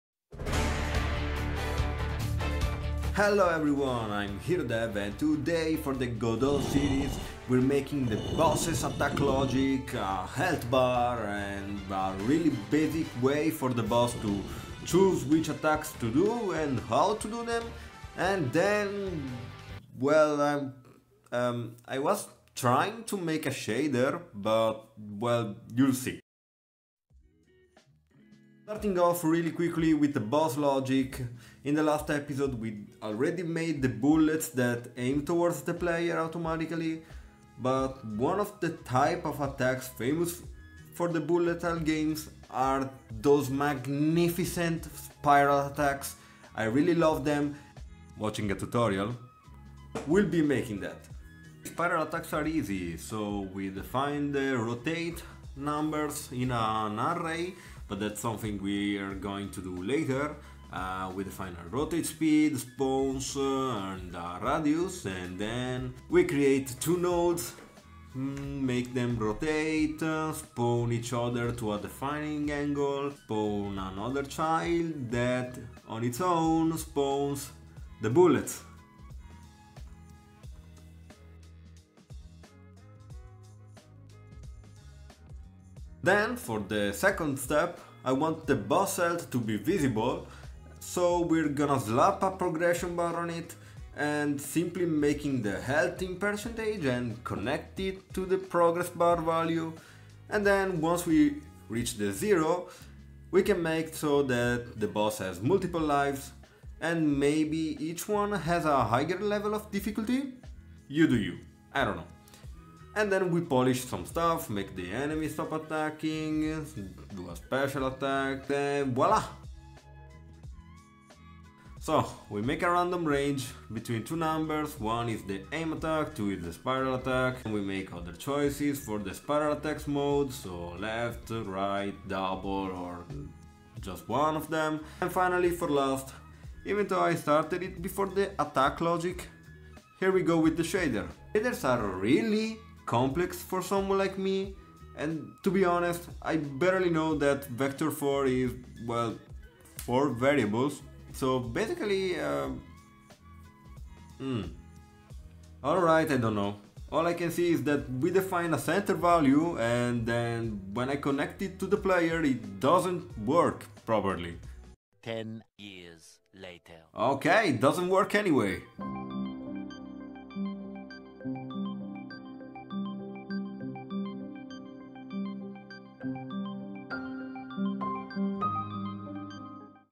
BOSSES IN GODOT?! HERE'S HOW I DID IT!|Godot Bullet Hell #2
Heyo people from the internet, today we're gonna look at a simple boss fight logic in GODOT ENGINE! What's next? You decide if you want!